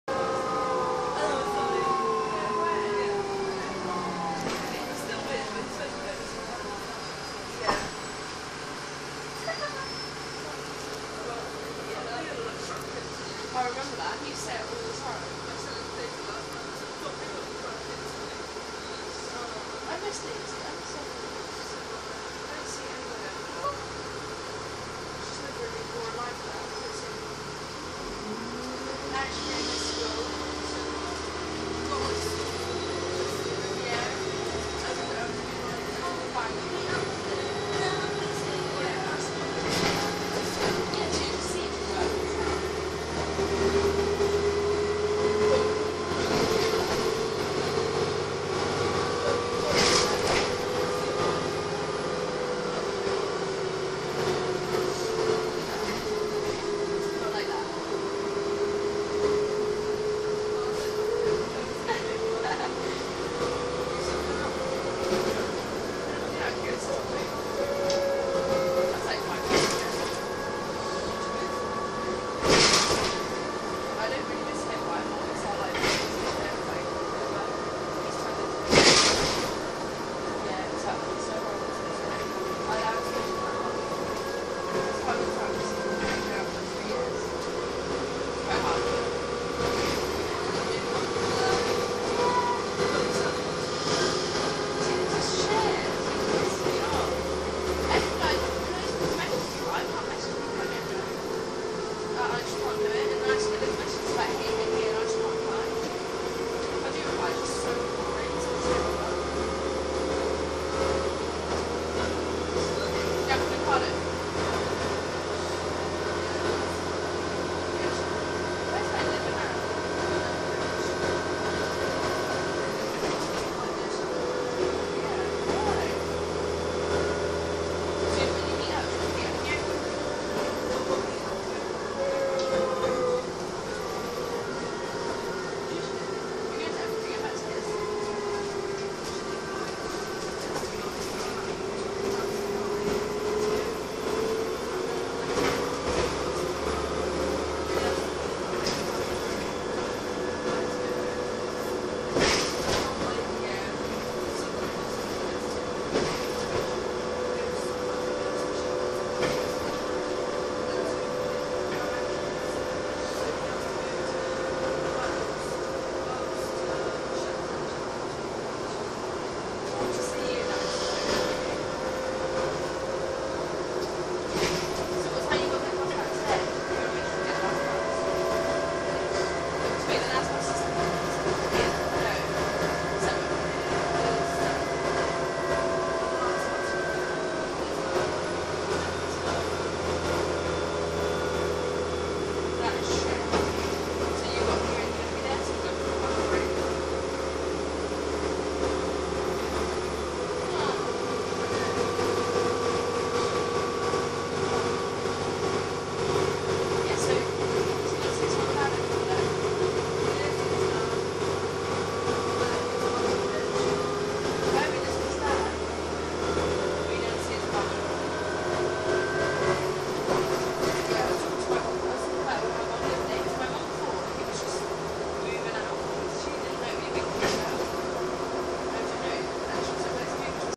Buses of Somerset 33381 LK53 EZA
Here we see a journey on Transbus/ALX400 Trident 33381 LK53 EZA seen on route 54 to Yeovil.